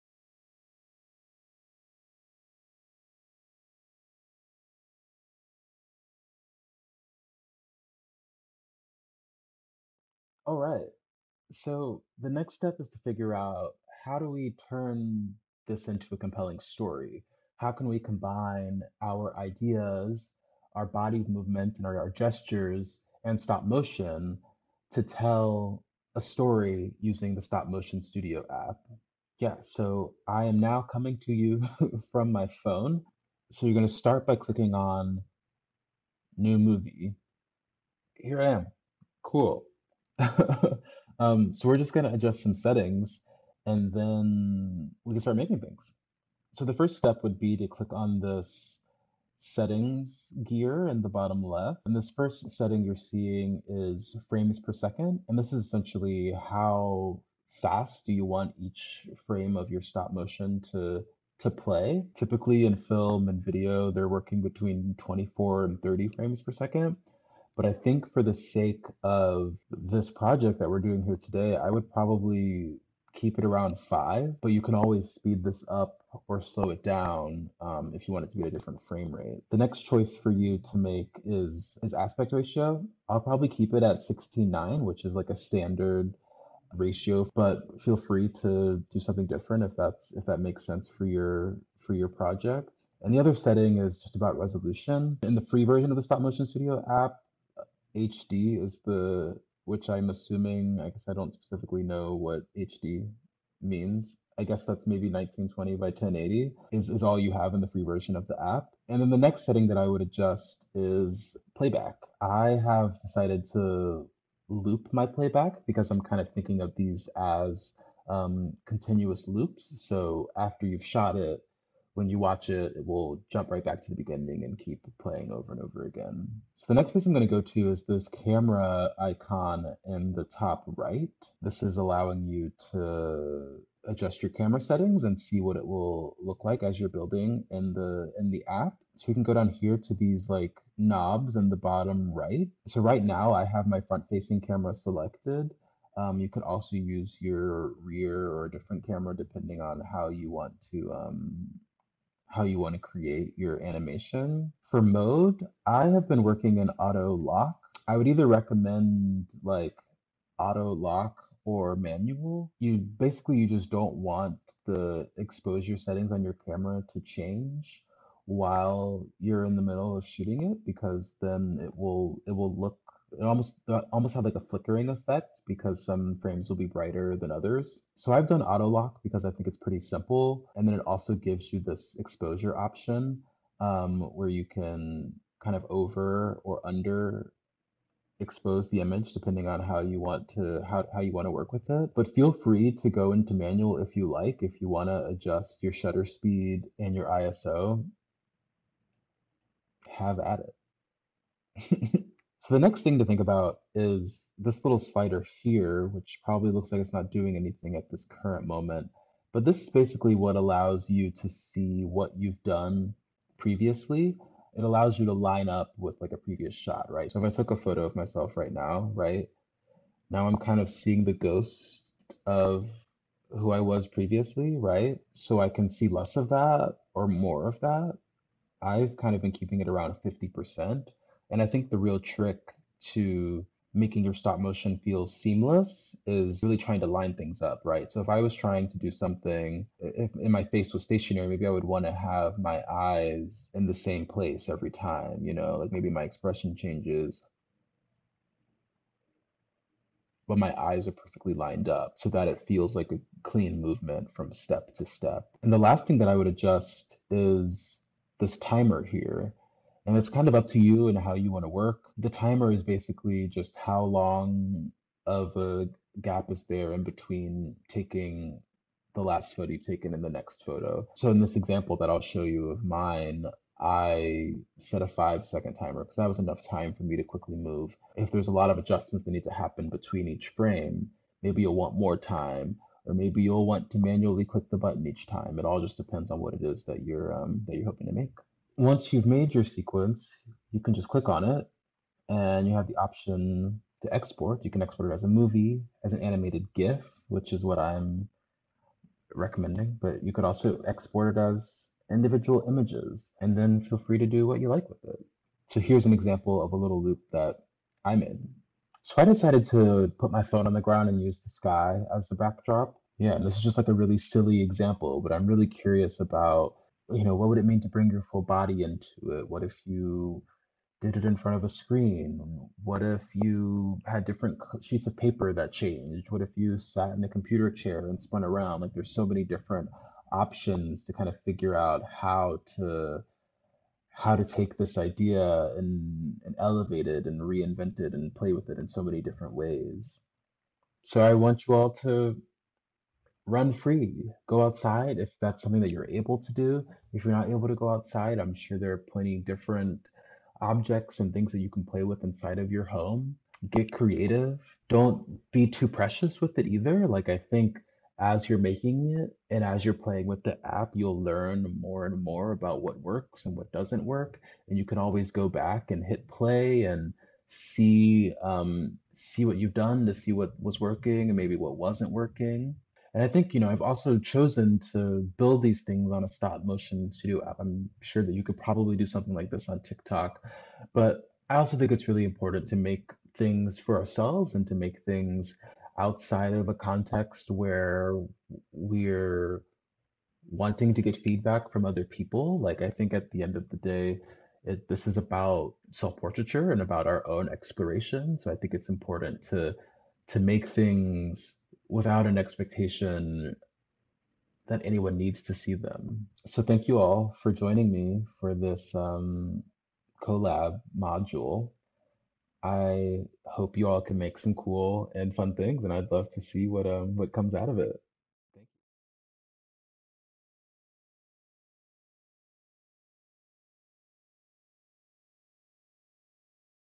CoLab | Stop-Motion Self-Portraiture | Create
In this module with Kameron Neal, we will look at the lineage of self-portraiture in the age of social media. Students will explore short-form storytelling by using their phones and their bodies to create kinetic stop-motion self-portrait .gifs.